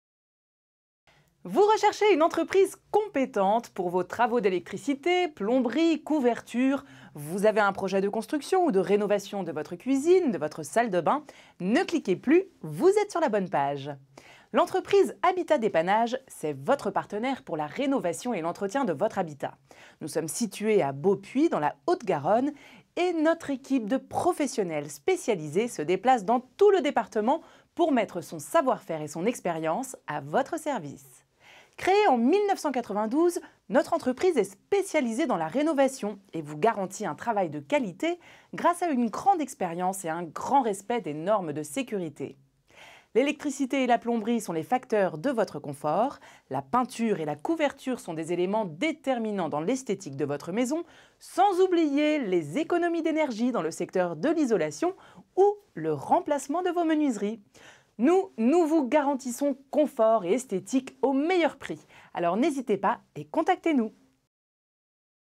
HABITAT DEPANNAGES : Electricité, Plomberie, Peinture, Couverture, Toulouse (31)
L'entreprise Habitat Dépannage, c'est votre partenaire pour la rénovation et l'entretien de votre habitat. Nous sommes situés à Beaupuy dans la Haute-Garonne et notre équipe de professionnels spécialisés se déplace dans tout le département pour mettre son savoir-faire et son expérience à votre service.
Créée en 1992, notre entreprise est spécialisée dans la rénovation et vous garantit un travail de qualité grâce à une grande expérience et un grand respect des normes de sécurité
L'électricité et la plomberie sont les facteurs de votre confort, la peinture et la couverture sont des éléments déterminants dans l'esthétique de votre maison, sans oublier les économies d'énergie dans le secteur de l'isolation ou le remplacement de vos menuiseries.
Nous, nous vous garantissons confort et esthétique au meilleur prix !! Alors n'hésitez pas et contactez-nous!